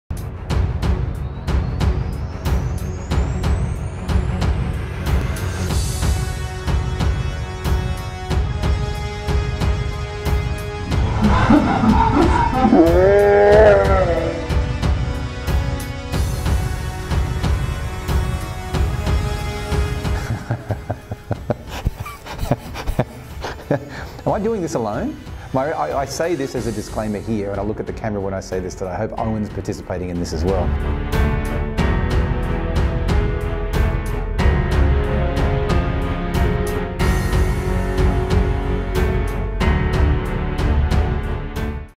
Charity teaser
Dare the director is back...

Last year Scott Smith ( Director) sacrificed himself for charity.

This year we are back again.